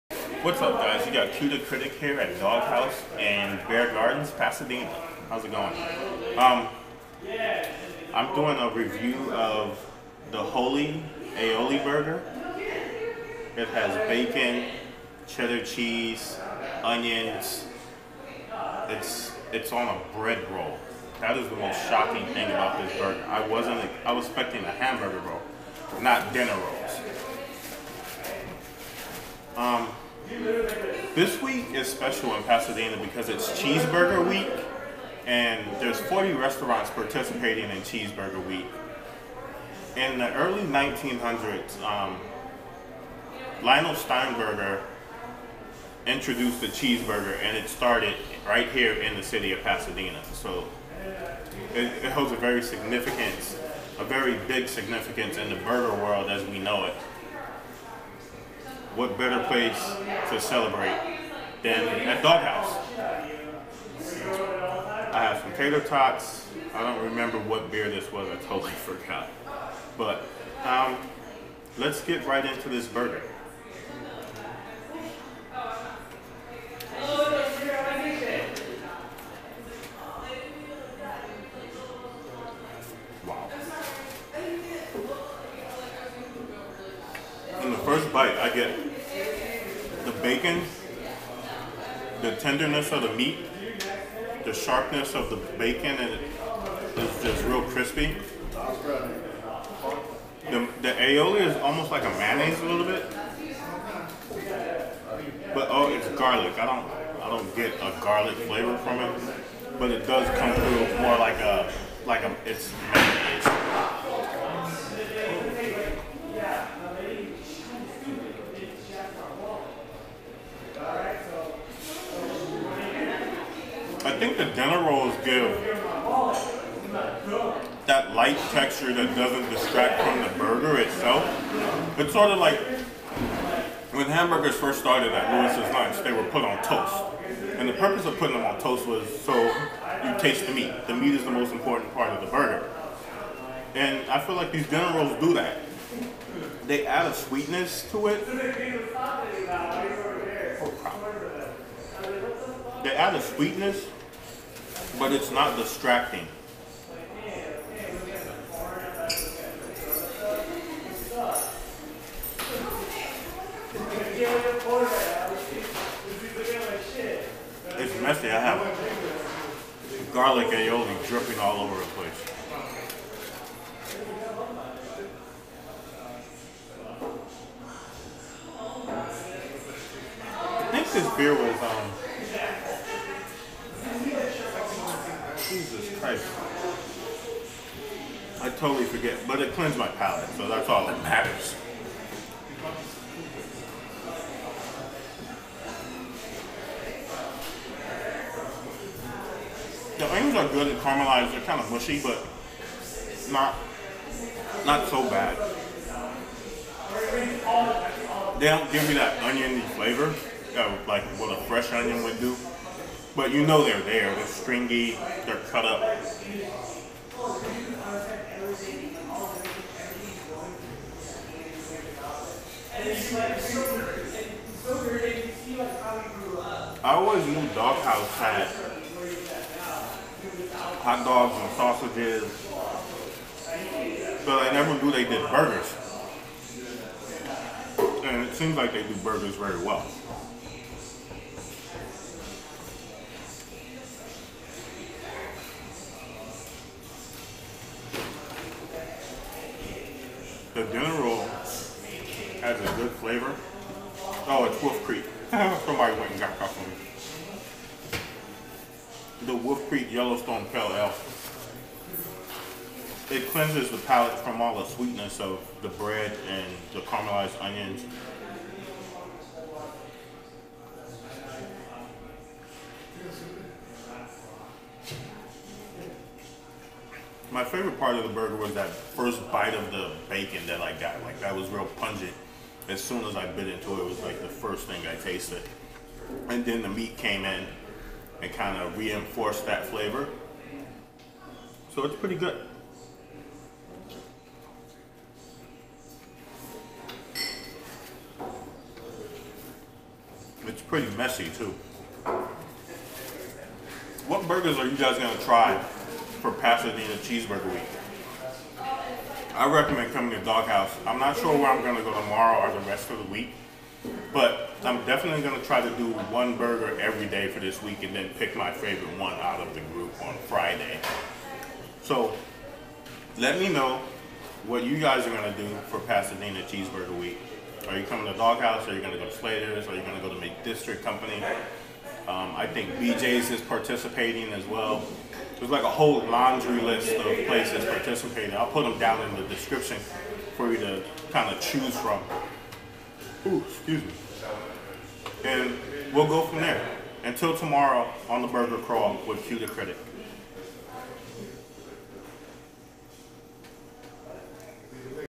Holy Aioli Burger at Dog Haus Biergarten | The Burger Crawl - Ep. 8
To Kick off Pasadena Cheeseburger Week 2015, the dinner rolls (bun) at Dog Haus excites me with their shocking presence.

Lionel Sternberger is credited with adding cheese to the hamburger sandwich in the 1926 at The Rite Spot.  Since then Pasadena has held its rightful place in the hearts and minds of burger lovers and burger historians.  Cheeseburger Week in is a week long celebration of the cheeseburger, and a remembering of the great history it has brung to burger culture, and to local Pasadena, CA.

 My first stop on the week long burger crawl is at Dog Haus Biergarten.  You are allowed to select any burger from their reasonably priced menu, and indulge.

Where will you go for Cheeseburger Week, maybe we can meet there?!

For more information on Pasadena Cheeseburger Week 2015, and the restaurants participating, please

Cue the Critic:  Food stories, Food Reviews, The Burger Crawl, Food-based events, Food Photography, Writer & "Cue the Critic" Quincy Freeman.